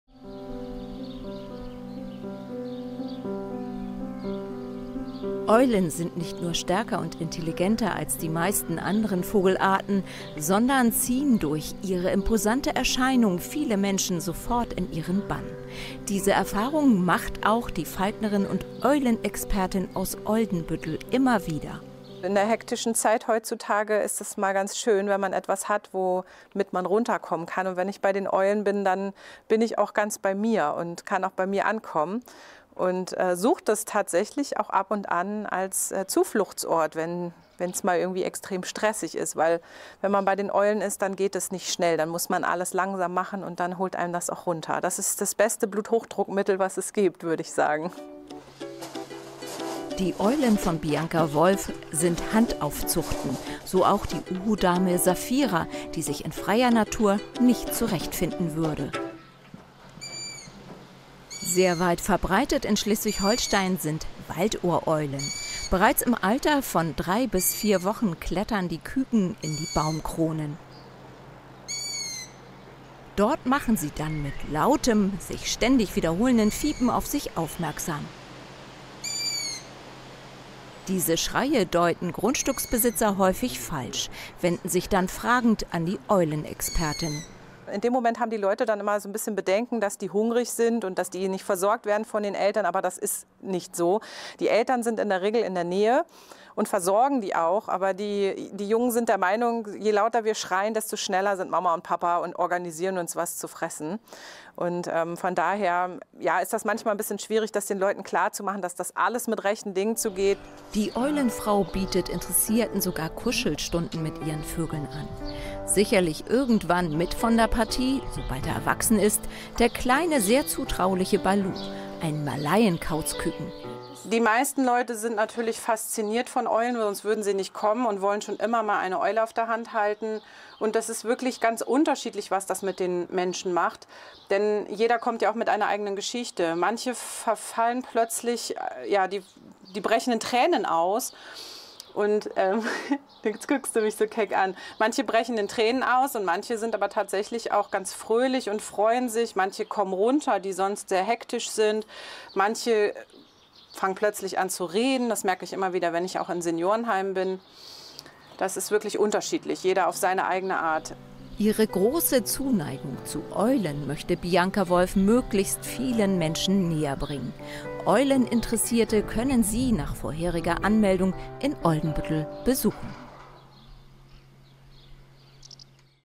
Eulenexpertin erklärt, warum Eulen gegen Stress und Hektik helfen
Eulen sind nicht nur stärker und intelligenter als die meisten anderen Vogelarten, sie ziehen durch ihre imposante Erscheinung auch viele Menschen sofort in ihren Bann. Diese Erfahrung macht Falknerin und Eulenexpertin Bianka Wolf immer wieder. Ihre große Zuneigung zu den Tieren möchte Bianca Wolf möglichst vielen Menschen näher bringen. Interessierte können sie nach vorheriger Anmeldung in Oldenbüttel besuchen.